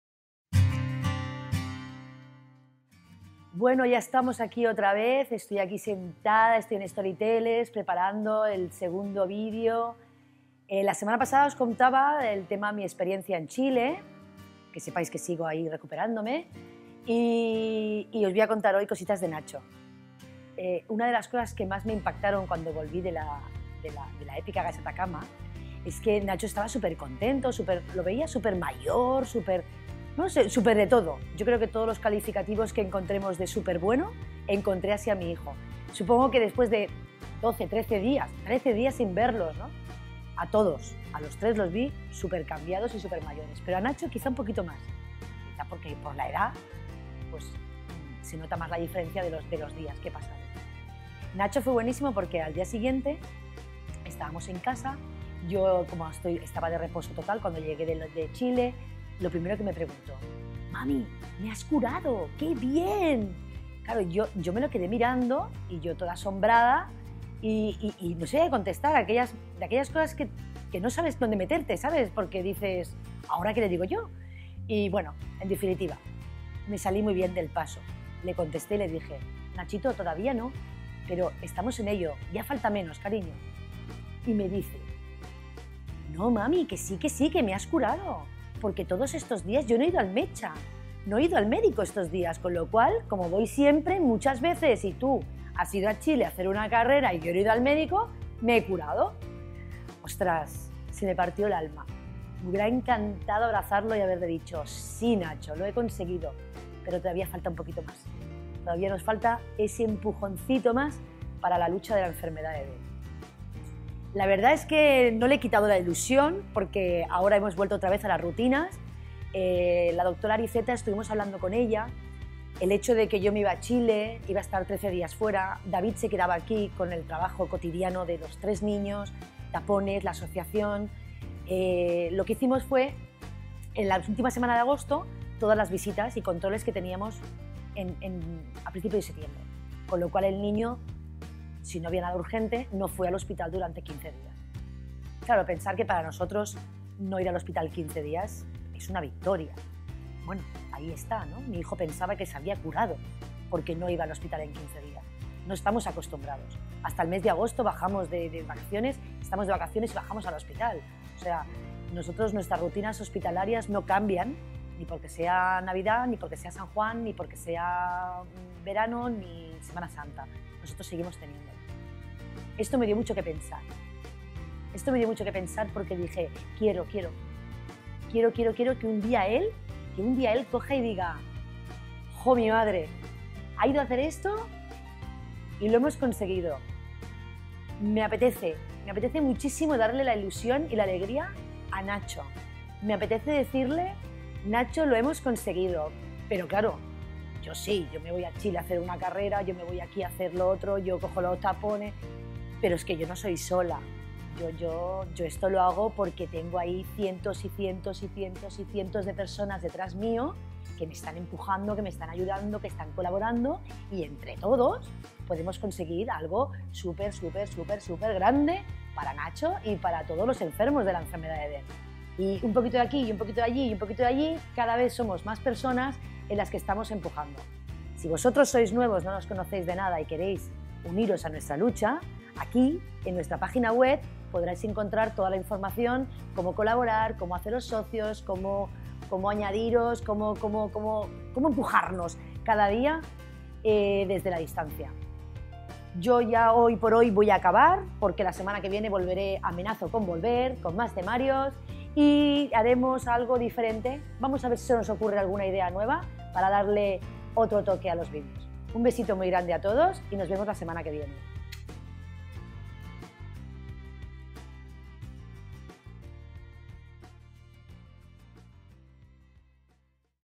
Eva nos habla de Nacho
En esta ocasión Eva nos habla de su hijo Nacho, enfermo de Dent.

Recuerda que, puntualmente, iremos publicando vídeos en el canal.